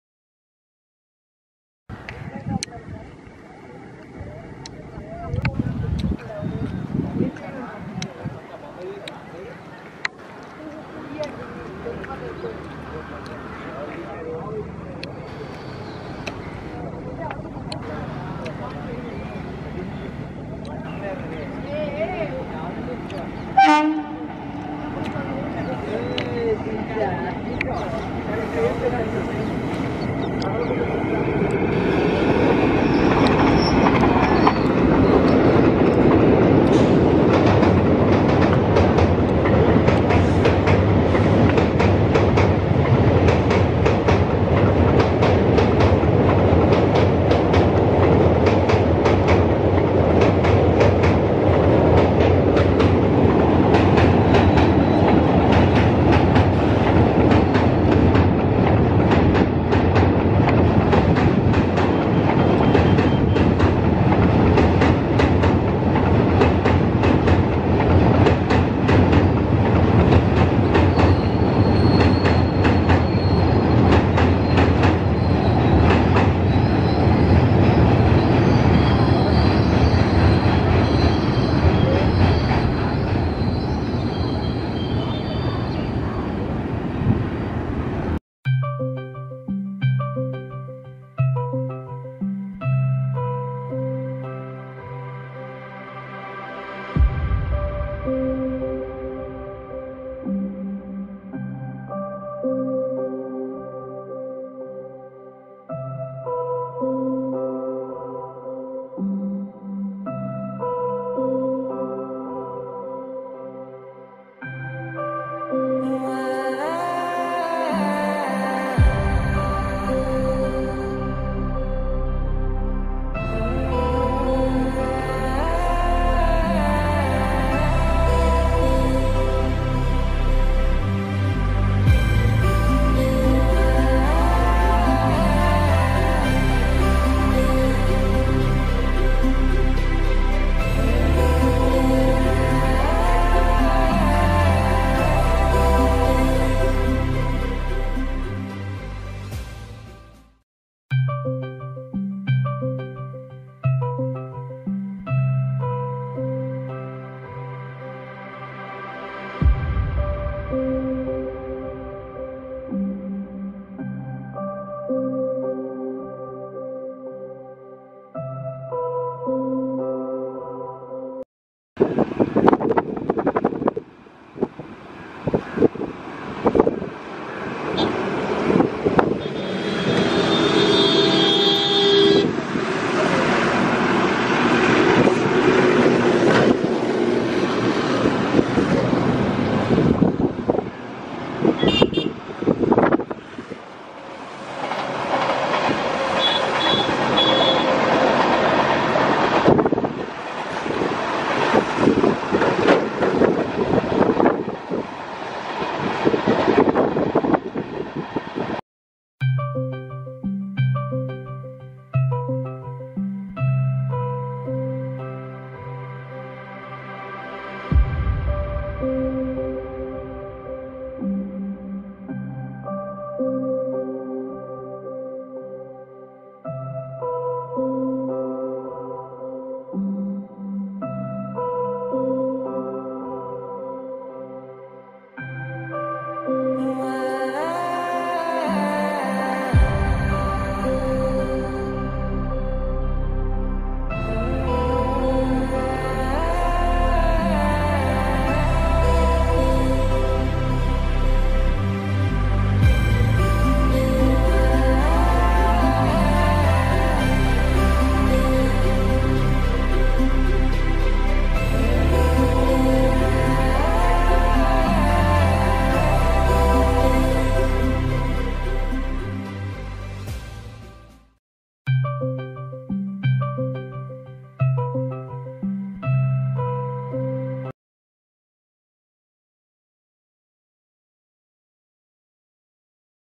Beautiful Hill Views | Konarak+ Kerala+ HWH - YPR Express | Indian Railways
Video Details:
1. 02641 | Thiruvananthapuram Central to Shalimar Covid -19 Special
Loco: WAP4 - Arakkonam - 22792
Location: Prakasam Barriage

2. 01019 | Konark Covid -19 Special
Loco: WAP4- Vijayawada - 22313
Location: Rayanapadu Workshop

3. 02873 | Howrah to Yesvanthpur Junction Covid -19 Special
Location: Gunadala

4. 02626 | Kerala Covid -19 Special
Newdelhi To Thiruvananthapuram
Location: Rayanapadu Work Shop

By: Venkatesh Tadiboina